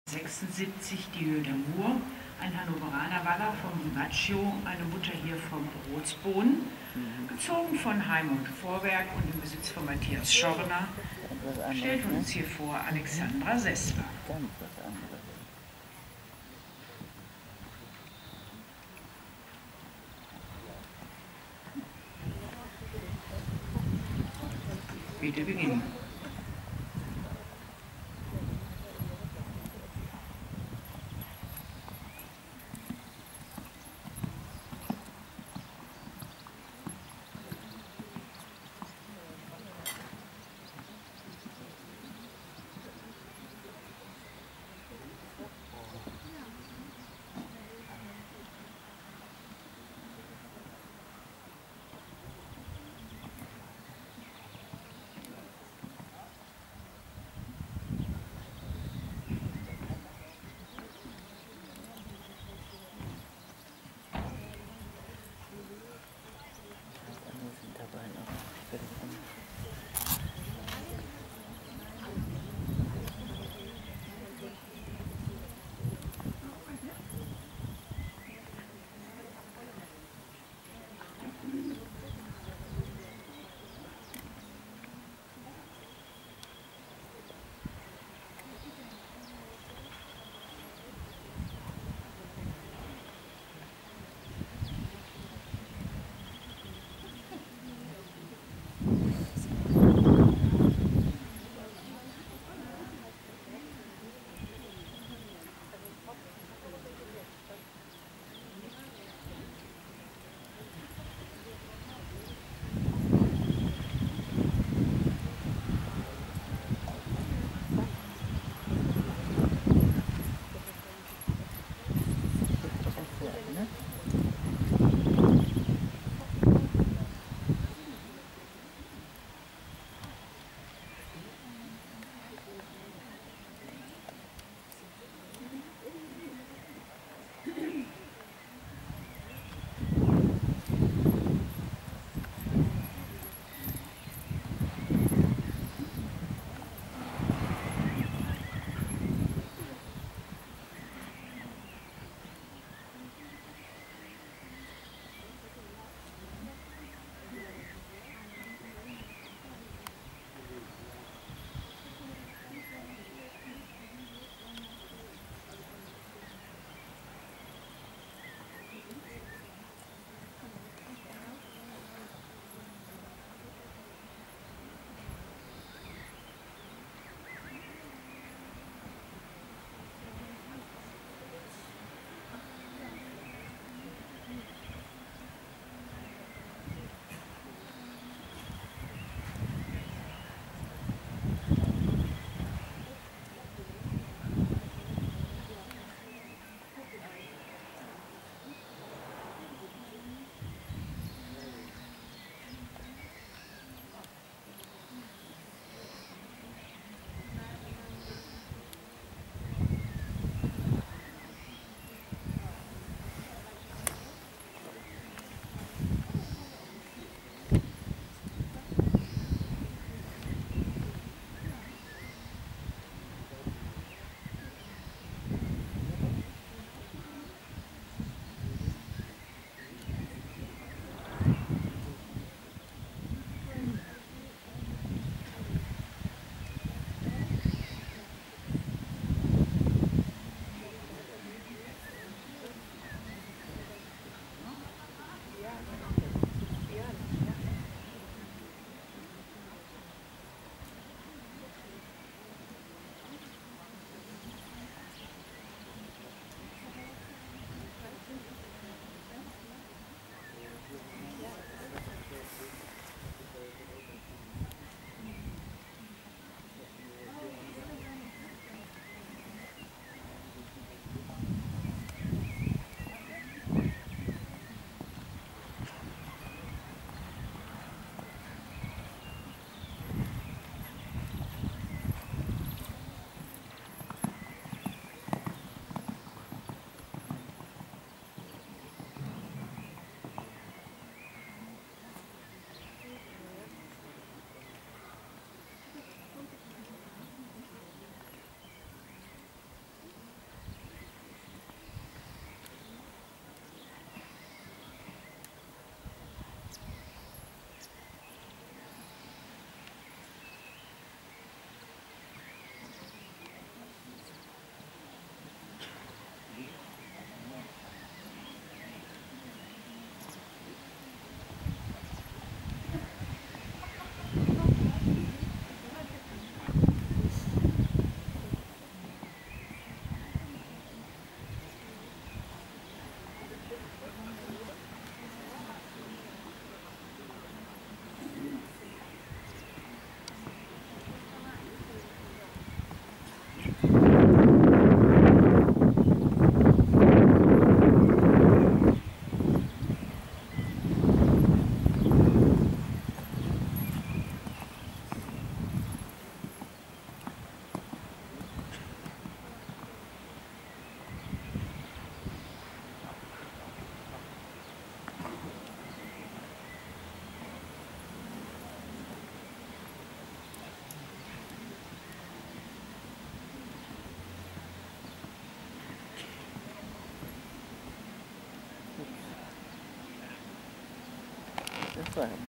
DIEU D'AMOUR & Alexandra Sessler - 2. Sichtung zur WM der Jungen Dressurpferde 2016
DIEU D'AMOUR - DbW., geb. Hann. 2009 v. Dimaggio x Rotspon
Z.: Raimund Vorwerk
Bes.: Matthias Schorner
Reiter: Alexandra Sessler

2. Sichtung mit Nominierung zur WM der Jungen Dressurpferde 2016